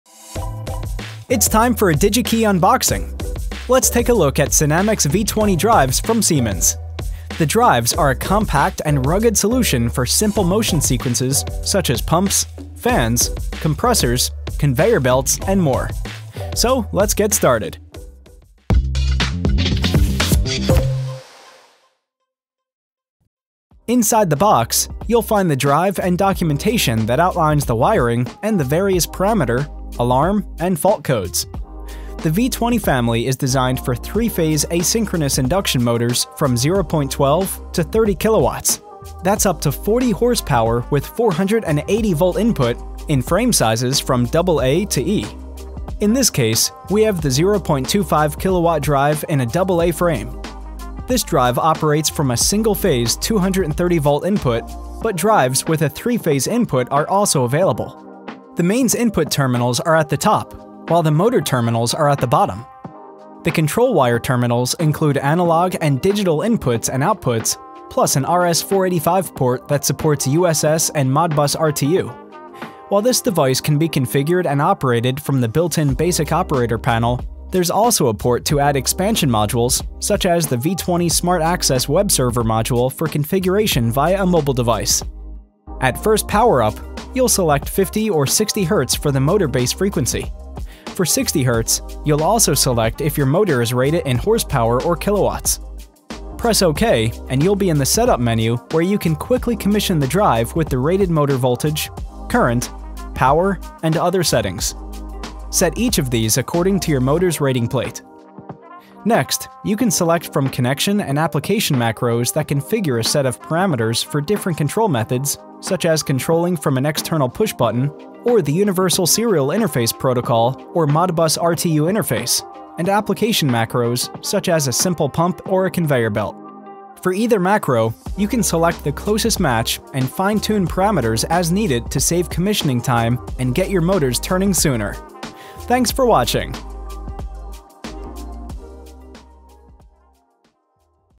SINAMICS V20 Drivers
Designed for three phase asynchronous induction motors from 0.12 to 30 kilowatts and up to 480 VAC input, in frame sizes from double-A to E.